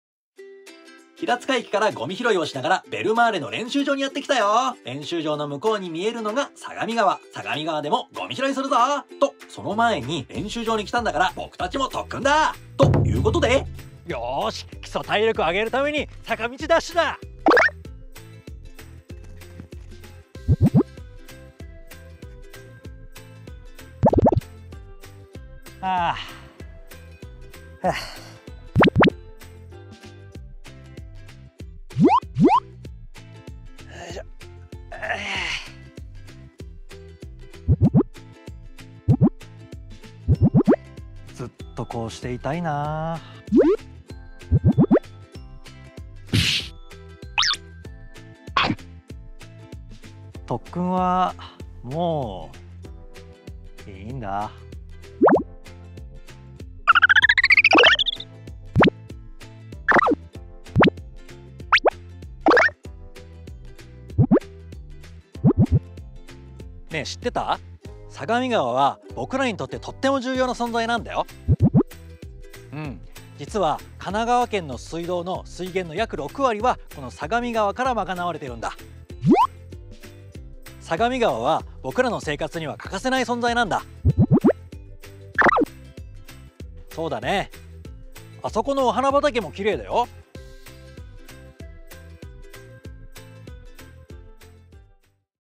湘南ベルマーレ LTO動画 ⑤
相模川のことを知るために、ベルマーレの練習場がある馬入に行ってきました！
と、その前にベルマーレの練習場前で特訓だー！

動画制作：ATSURAELU（アツラエル）
◆LTO（湘南ベルマーレとやっているゴミ拾い）